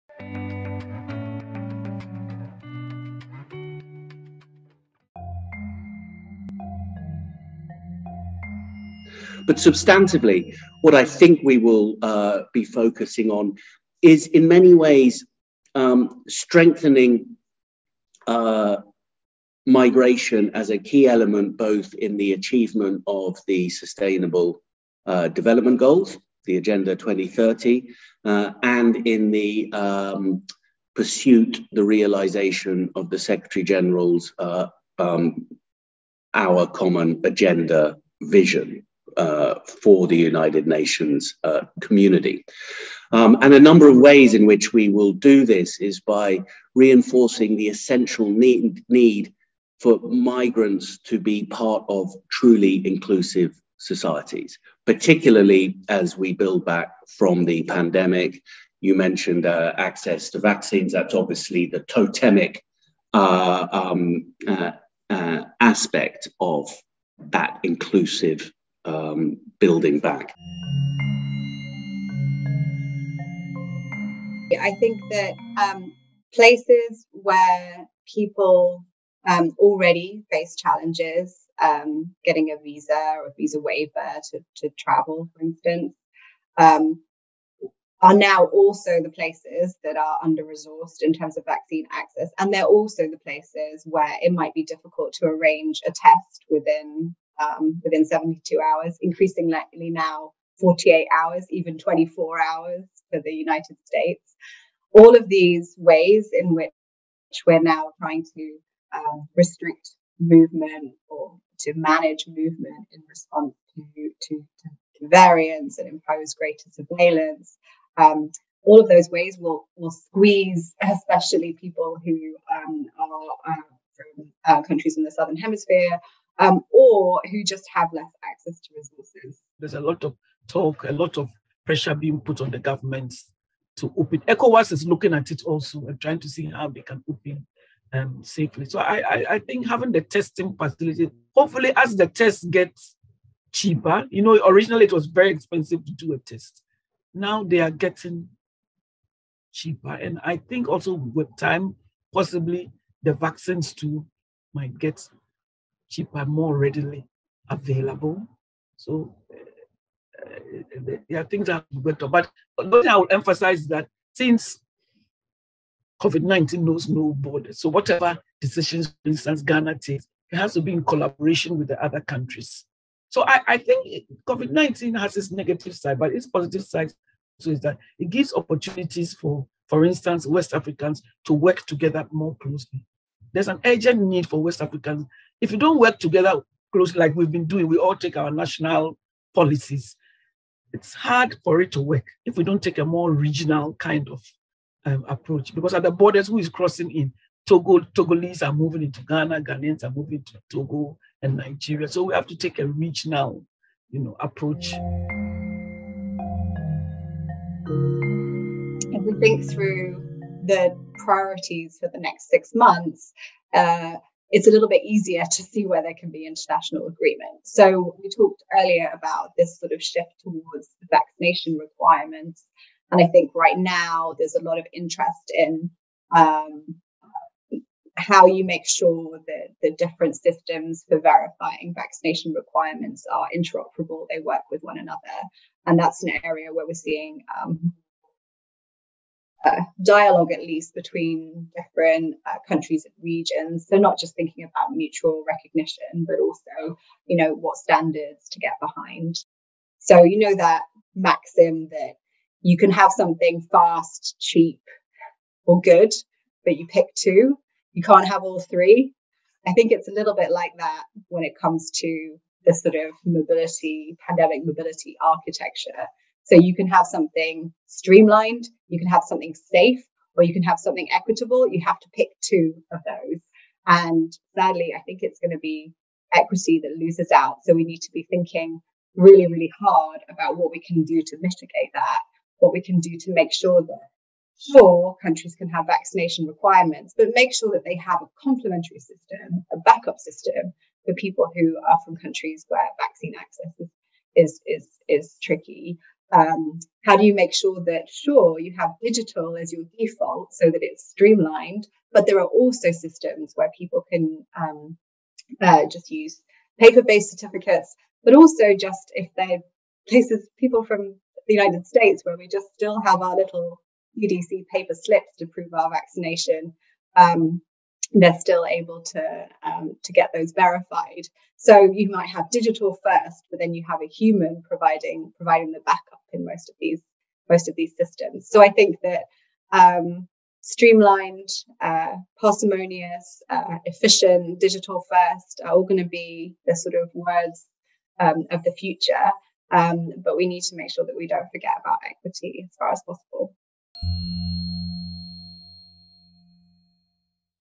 "Une reconstruction inclusive"
Entretiens avec :
- Meghan Benton, directrice de la recherche internationale, Migration Policy Institute
- Jonathan Prentice, responsable du secrétariat, Réseau des Nations unies sur les migrations
- Mariama Awumbila, professeure associée, Université du Ghana

(Entretiens menés entre décembre 2021 et janvier 2022)